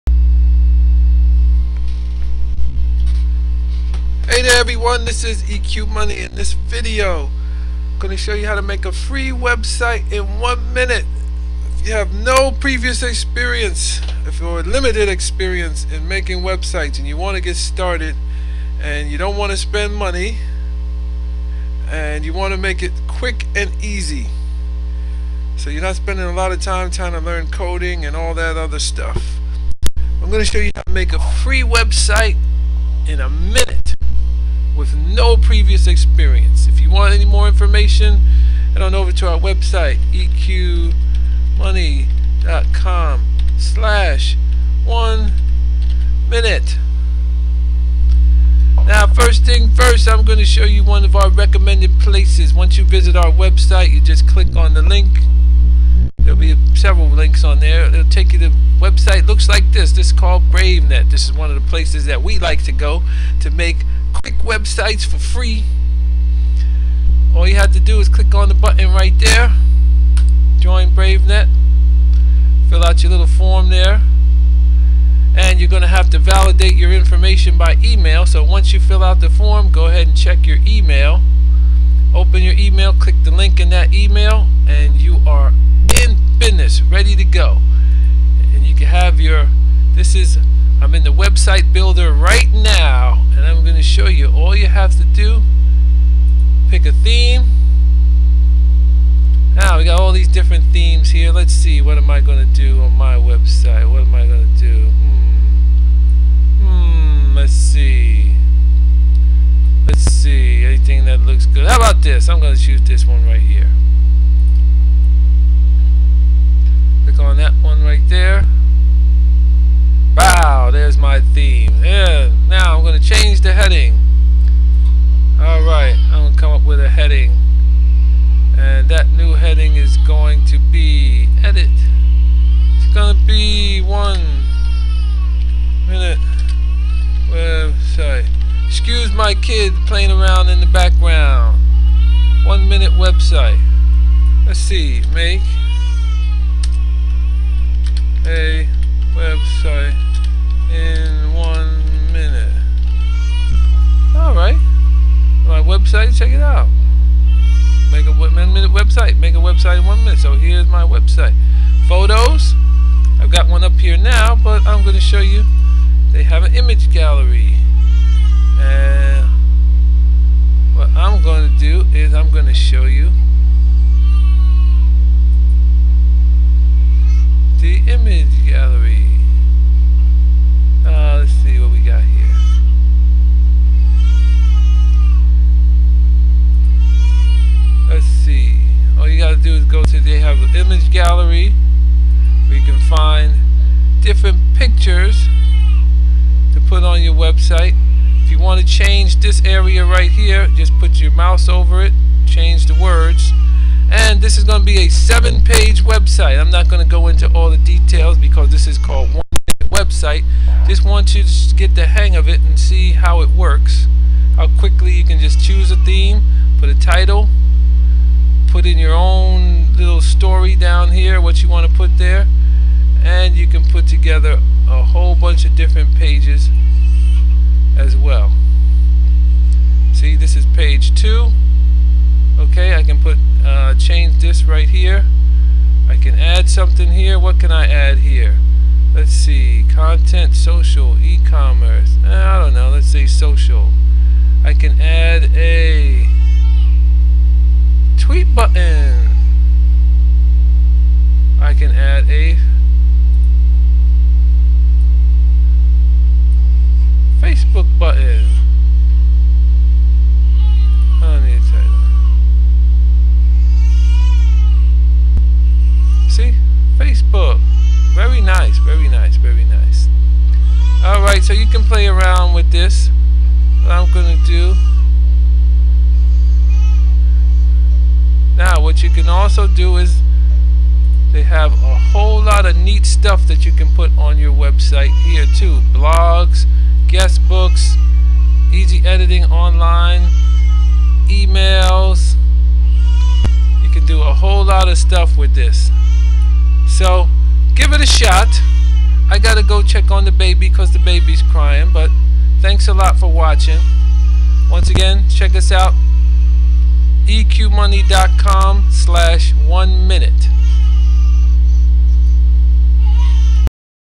Build a Free Website in Minutes
Build a Free website in minutes with no previous experience. Newbie friendly.